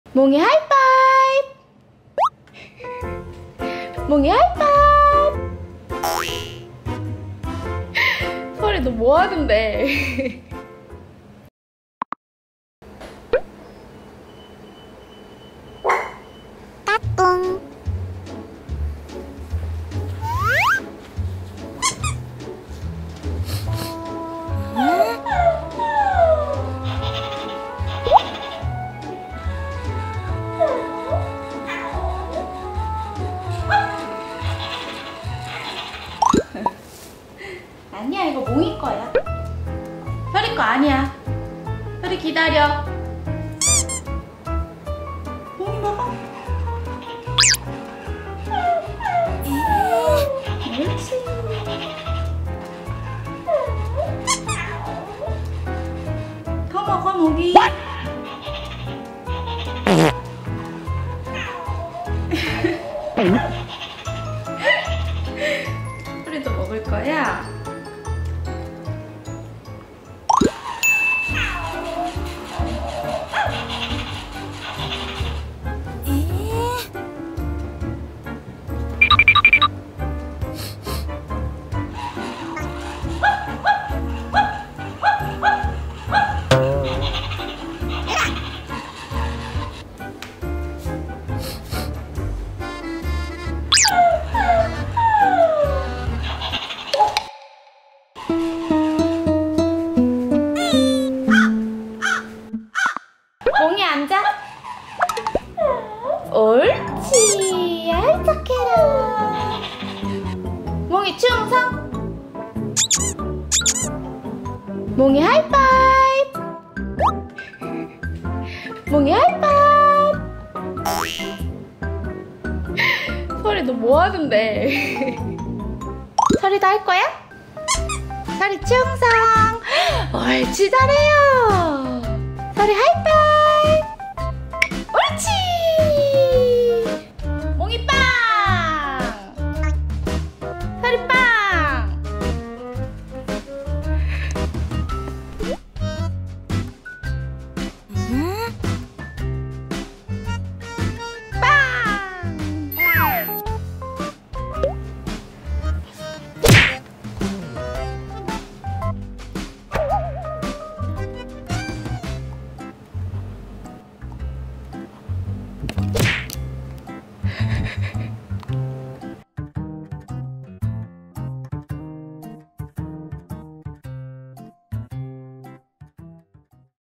외동으로 살던 강아지에게 동생을 만들어주면 생기는 일
설이야...누나 마음 속 1번은 언제나 너야ㅠㅠ
누나 마음 알지??(뽑뽀백번)

Profile💛
Name : Hong seol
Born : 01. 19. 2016
Breed : White Pomeranian(♂ )
Favorite Food : Yogurt&Cheese